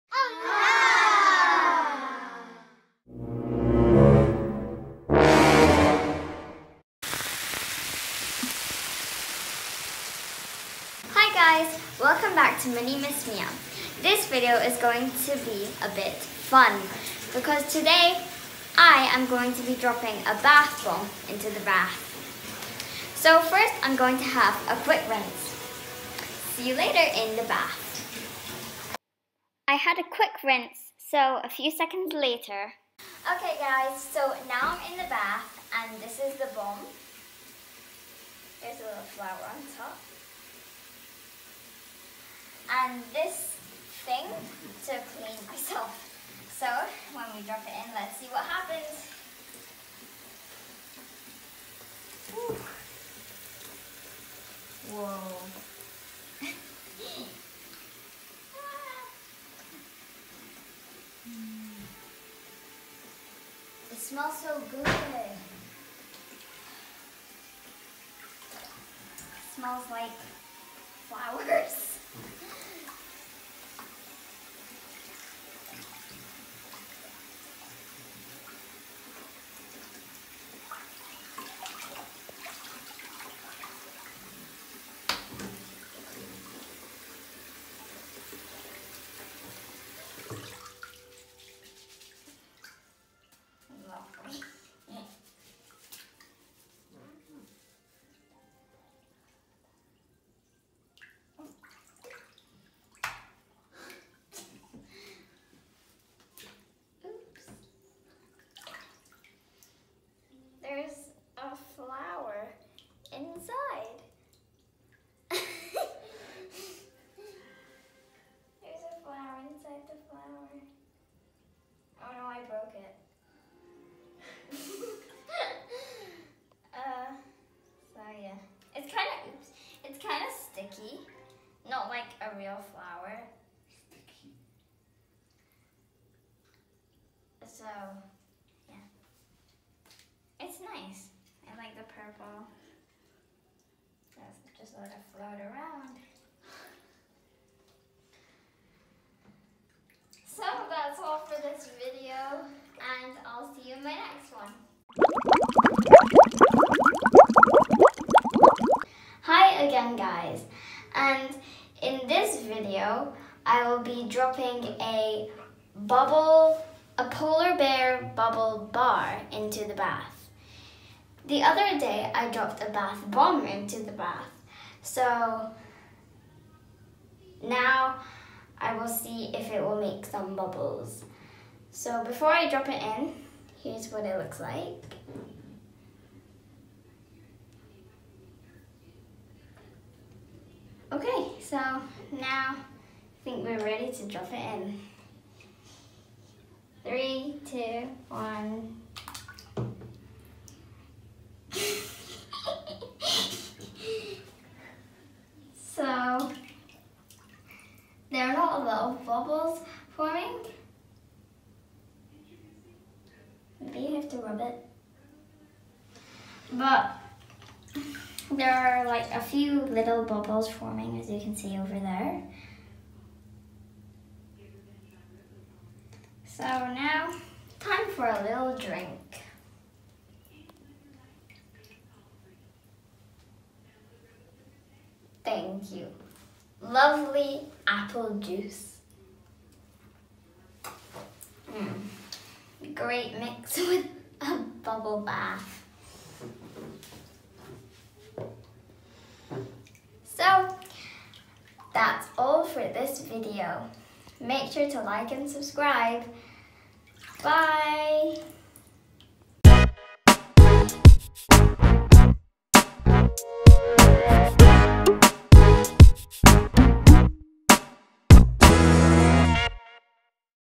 Bath Bomb vs. Bubble Bar / Mini Miss Mia
Hi guys!

Today's video will be a bit fun... I will be testing a bath bomb and a bubble bath bar! I hope you enjoy this video.

This is not a sponsor, but I would just like to thank LUSH for supporting everyone and selling AMAZING bath cosmetics and more!

If you want to watch some of my other videos, here are some links: